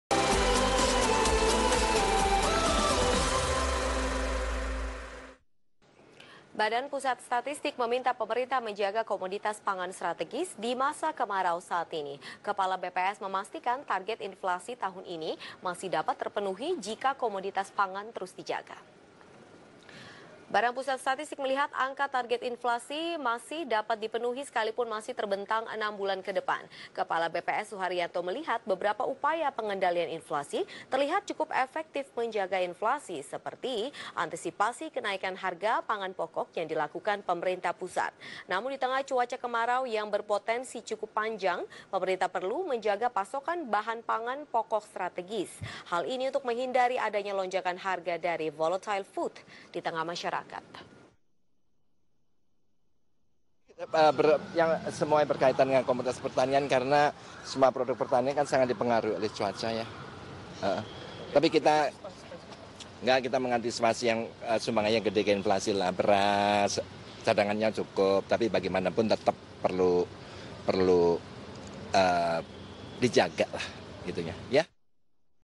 BPS SEBUT UPAYA PENGENDALIAN INFLASI CUKUP EFEKTIF
IDX CHANNEL - Badan Pusat Statistik (BPS) meminta pemerintah menjaga komuditas startegis di musim kemarau saat ini. Kepala BPS memastikan target inflasi tahun ini masih dapat terpenuhi jika komuditas pangan terus dijaga.